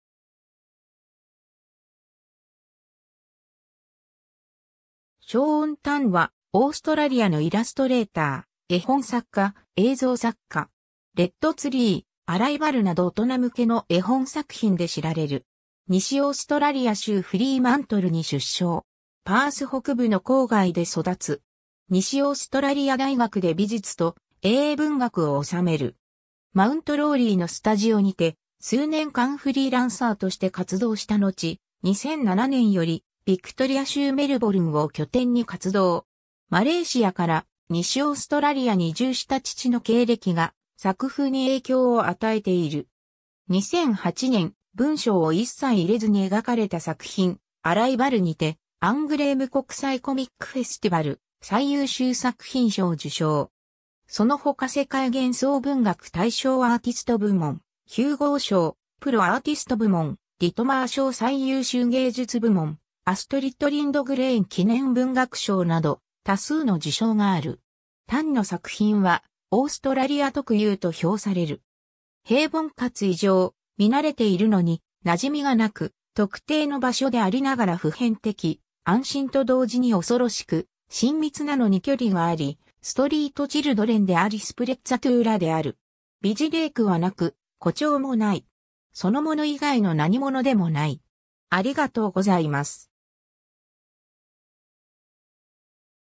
ショーン・タン
アストリッド・リンドグレーン記念文学賞受賞者
ショーン・タン（Shaun Tan、1974年 - ）は、オーストラリアのイラストレイター、絵本作家、映像作家。
『レッドツリー』『アライバル』など大人向けの絵本作品で知られる。
 西オーストラリア州フリーマントルに出生。
パース北部の郊外で育つ。
西オーストラリア大学で美術と英文学を修める。
マウント・ローリーのスタジオにて数年間フリーランサーとして活動したのち、2007年よりビクトリア州メルボルンを拠点に活動。
マレーシアから西オーストラリアに移住した父の経歴が作風に影響を与えている。
2008年、文章をいっさい入れずに描かれた作品（グラフィック・ノベル）『アライバル』にてアングレーム国際コミック・フェスティバル最優秀作品賞受賞。
そのほか世界幻想文学大賞アーティスト部門、ヒューゴー賞プロ・アーティスト部門、ディトマー賞最優秀芸術部門、アストリッド・リンドグレーン記念文学賞など多数の受賞がある。
 タンの作品は「オーストラリア特有」と評される。
「平凡かつ異常、見慣れているのになじみがなく、特定の場所でありながら普遍的、安心と同時に恐ろしく、親密なのに距離があり、ストリートチルドレンでありスプレッツァトゥーラである。美辞麗句はなく、誇張もない。そのもの以外の何物でもない」。